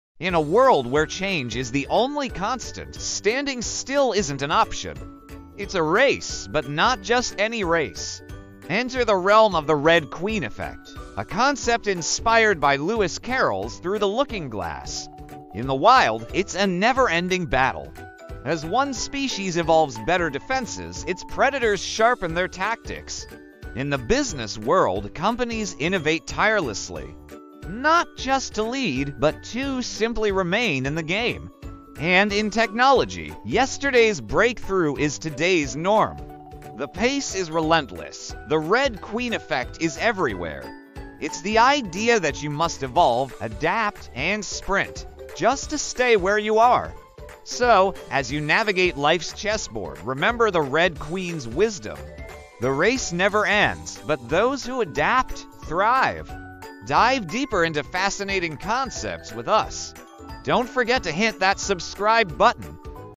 The Red Queen Effect: How to Stay Ahead of the Competition
Explore the intriguing concept of the Red Queen Effect, a phenomenon inspired by Lewis Carroll's 'Through the Looking-Glass'. Dive deep into the relentless race of evolution in nature, business, and technology. Whether it's species adapting in the wild, companies innovating to stay ahead, or the rapid pace of tech advancements, the Red Queen Effect is omnipresent. This video sheds light on why standing still means falling behind and how constant adaptation is the key to survival. If you're curious about evolutionary biology, business strategies, or technological trends, this is a must-watch.